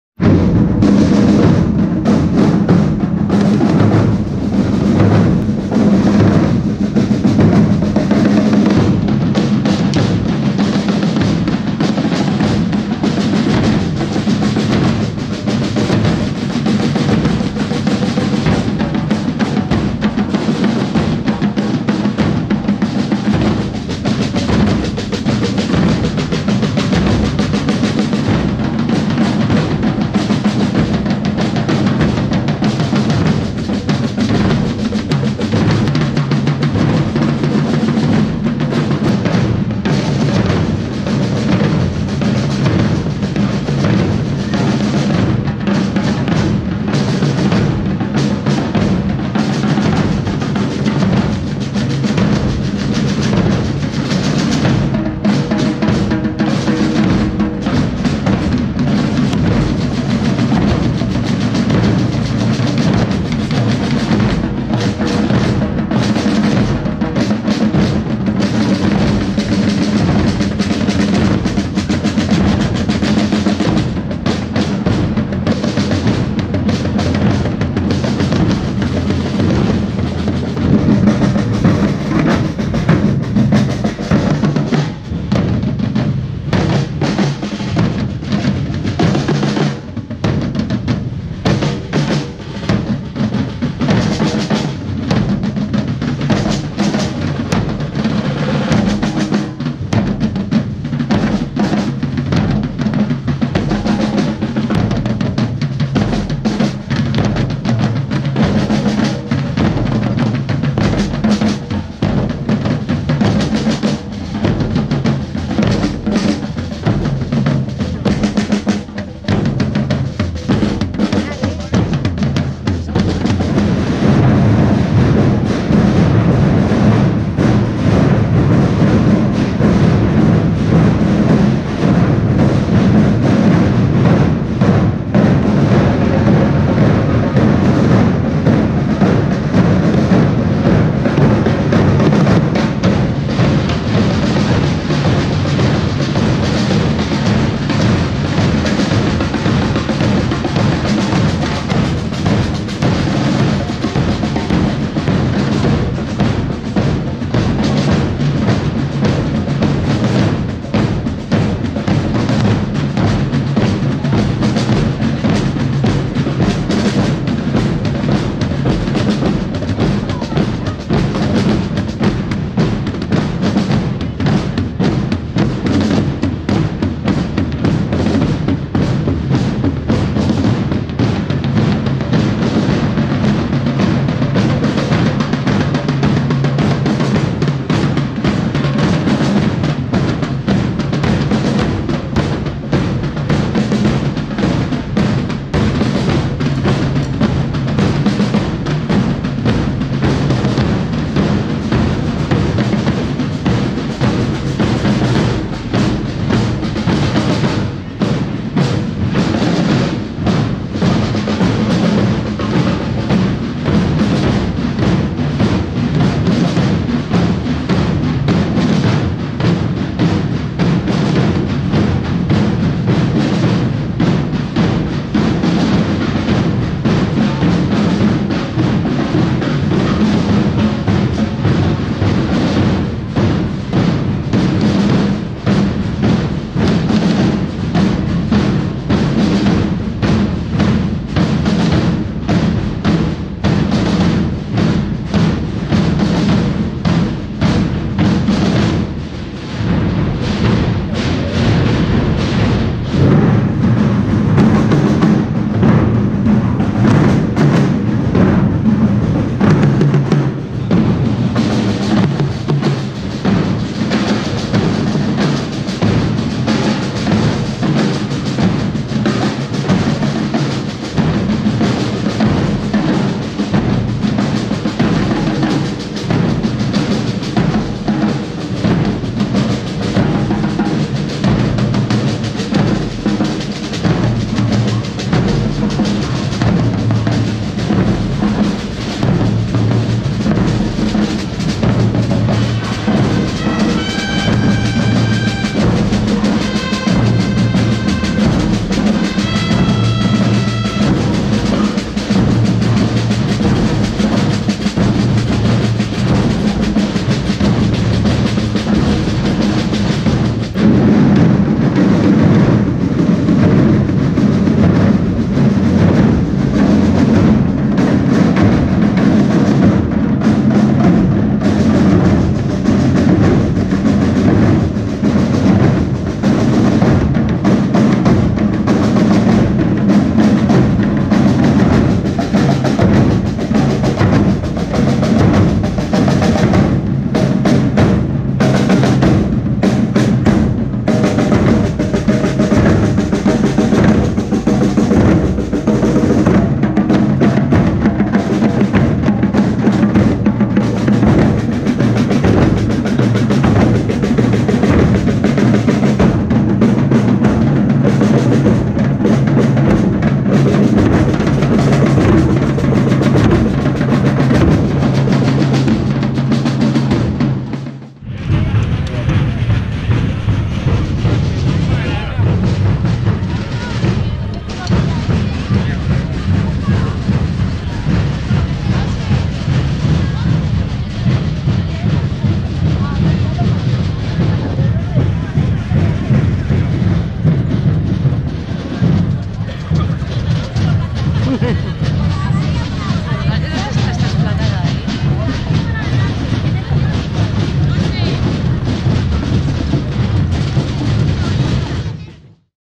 V ENCUENTRO DE BANDAS EN JACA (HUESCA)
La Cofradía Veracruz de Alfaro participa en este encuentro de bandas por 2º año consecutivo, esta vez el tiempo acompañó durante toda la jornada de este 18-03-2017.